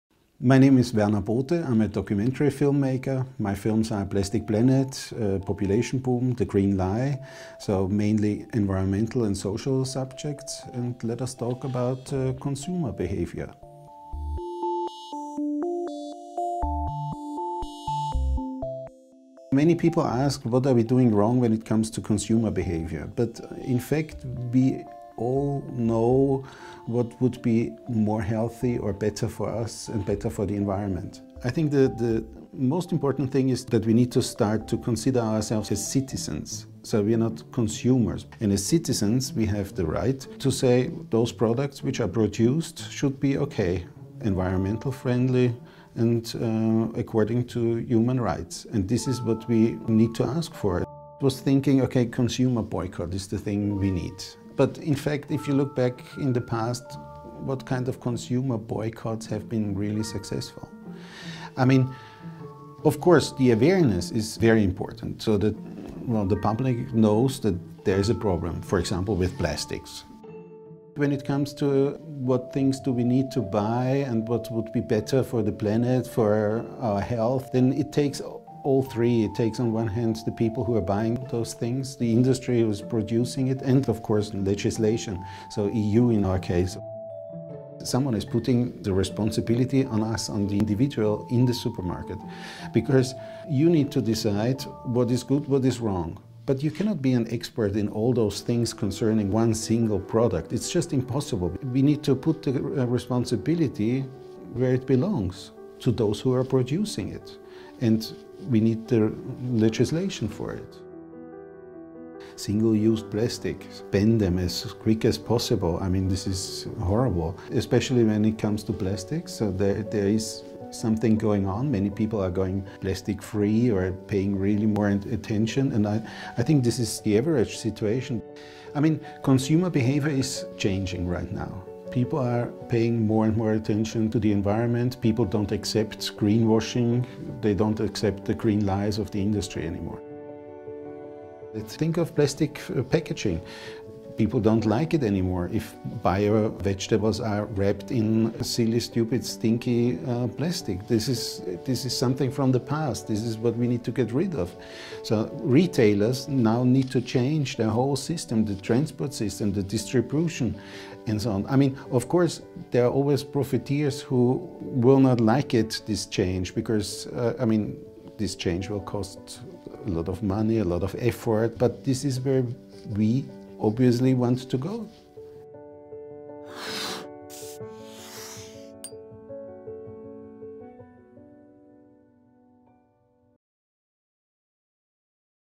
Werner Boote about Consumer Behaviour
Consumer behaviour is changing - people refuse to buy packaged vegetables, bring their own bags on shopping trips and order their drinks without straws. But is it only the individual's responsibility to act on climate change? Werner Boote, director of "Plastic planet", about consumer behaviour.